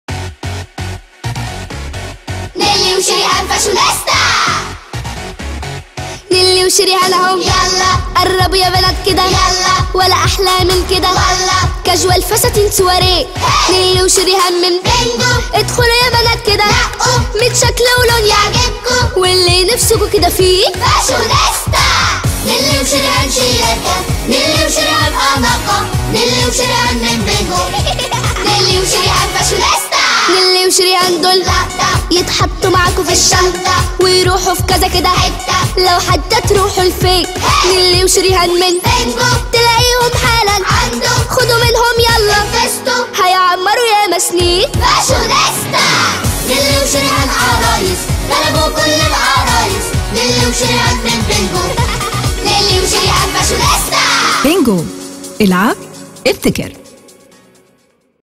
Bingo Fashionista ( Nilly & sherihan ) - نيلي و شريهان فاشونستا
عروسة نيلى و شيريهان فاشونيستا 14 شكل مختلف.
العروسة لها 10 مفصلات فى جسمها عشان تتحركبسهولة.
بيجى معاها اكسسوارات: حلق, عقد, شنطة, جزمة, مشط و فرشة.
جمع الاشكال كلها.

14 Designed
the doll has 10 joints for flexible movement of her arms and legs and comes with different accessories (Bag, Shoes, Earrings, Necklace, and hair brush and comb)